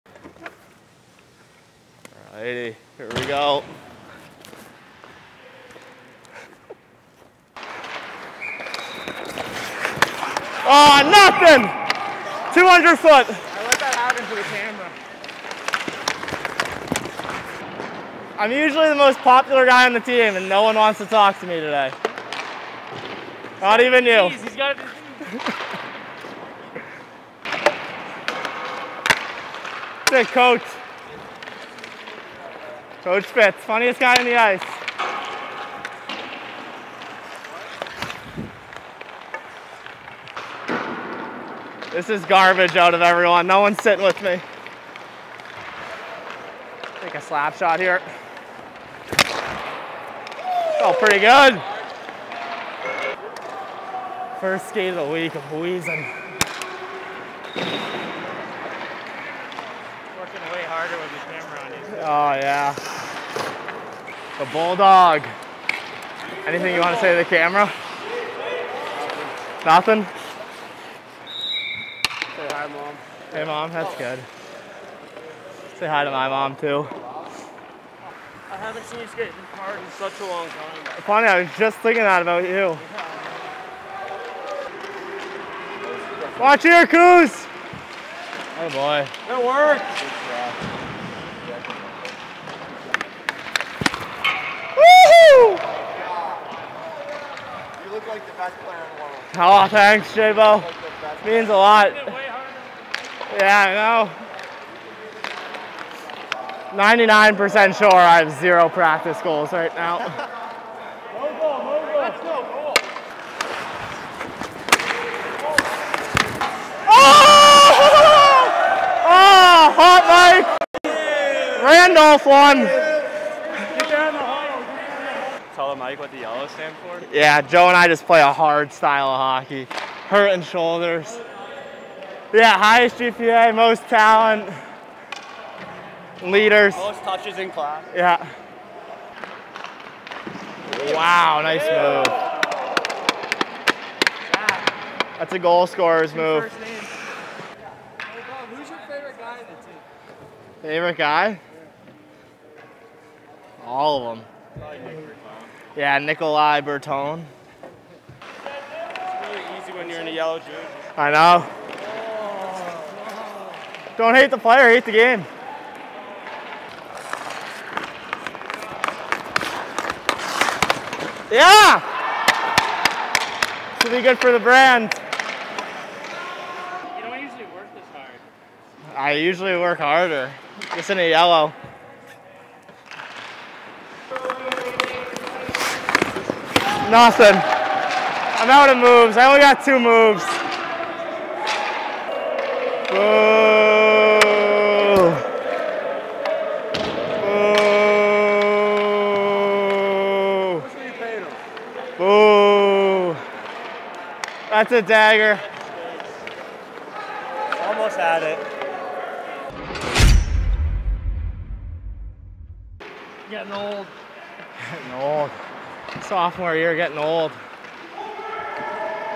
Mammoths Mic'd Up: Bobby Luca, Men's Ice Hockey
Bobby Luca of the Amherst College men's ice hockey team is mic'd up for practice!